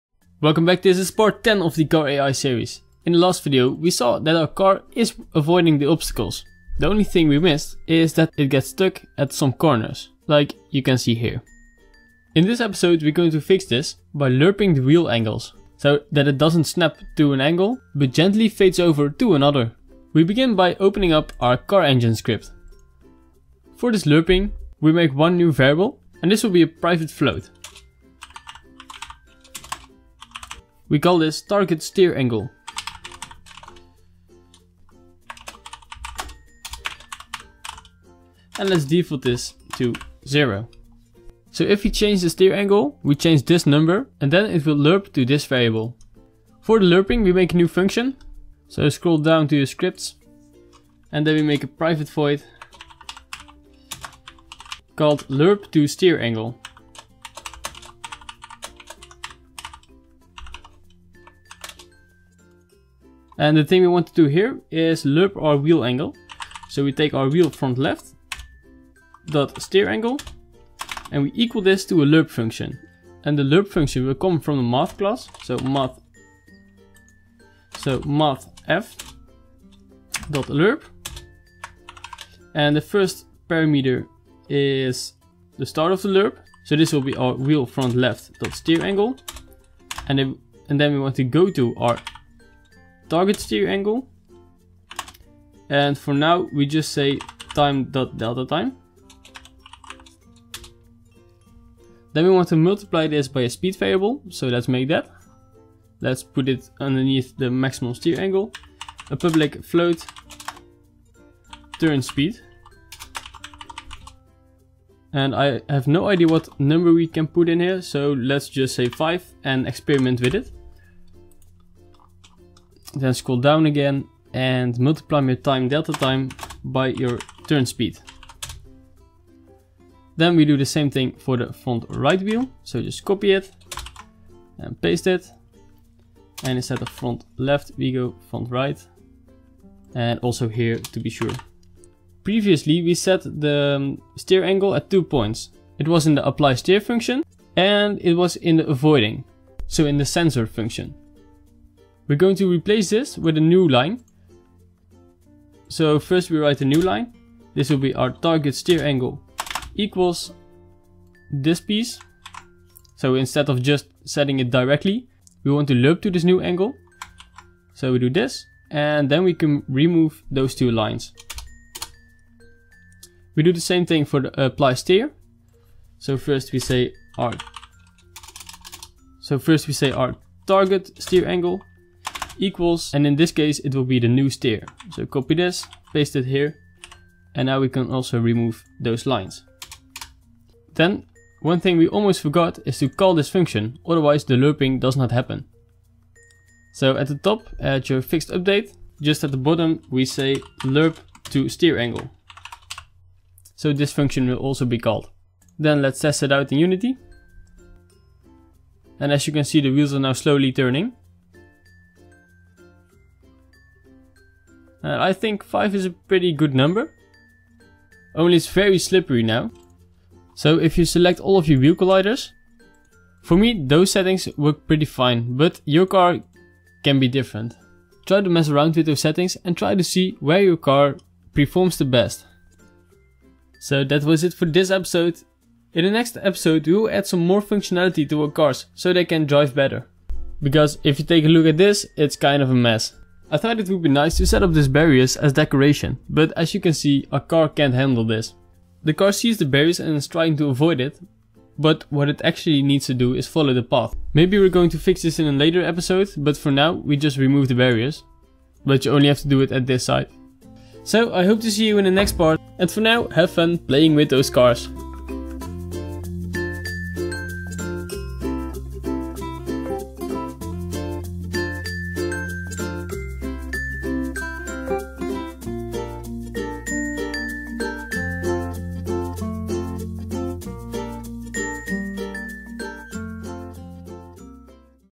Car AI Tutorial #10 (Unity 5) - Smoothing Turns
Smoothing the wheel angles to make smoother turns